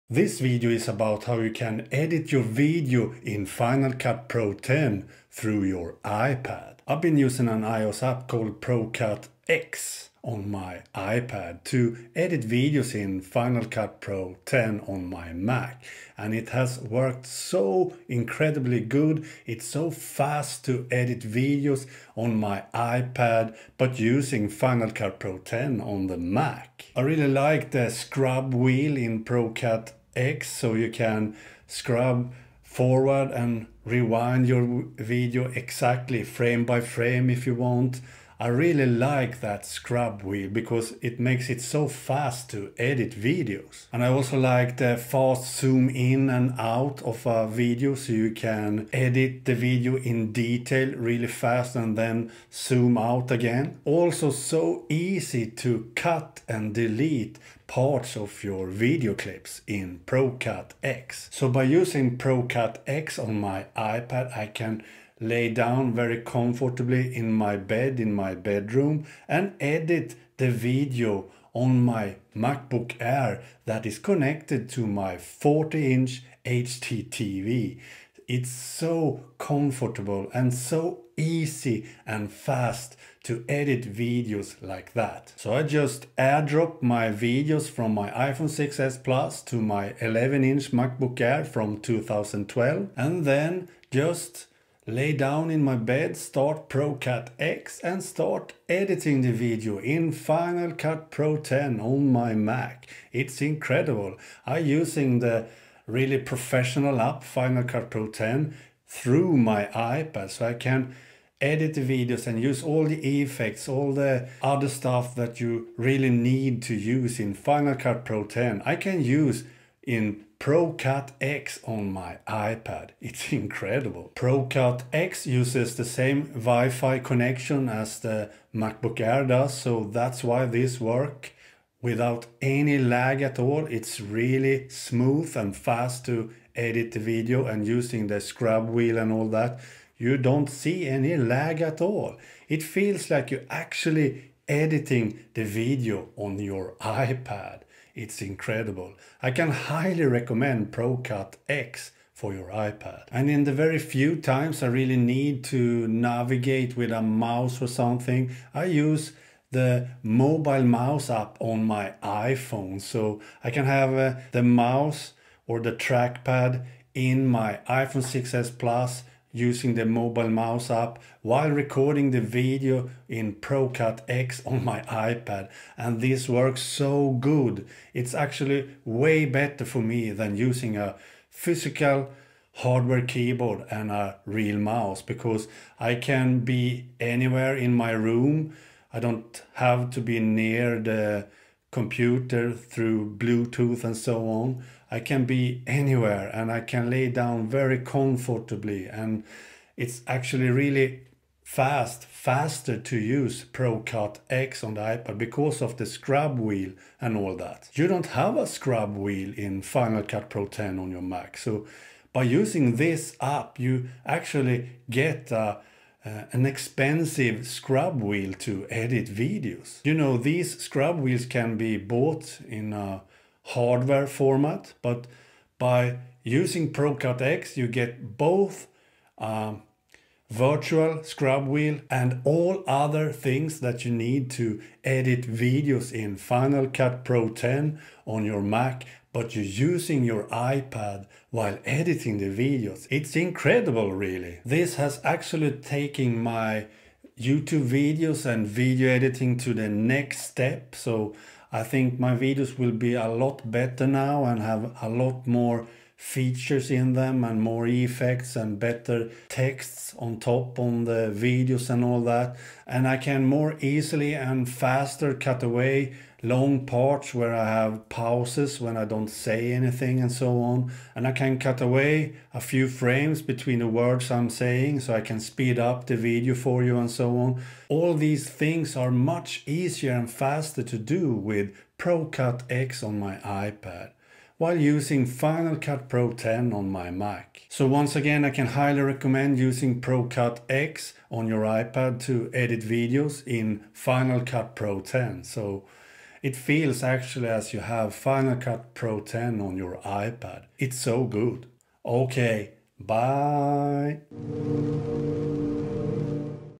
Using your iPad when editing videos in Final Cut Pro X
I have found a way to edit videos with my iPad while I'm actually using Final Cut Pro X. And it's an incredibly good and effective workflow for me. Watch this video to see how!

Gear used:
iPhone 6s plus (recording)
iPad Air 2 (editing)
Rode smartlav+ or Shure MOTIV MV5 microphones (recording)
iMovie (editing)
Over (thumbnails)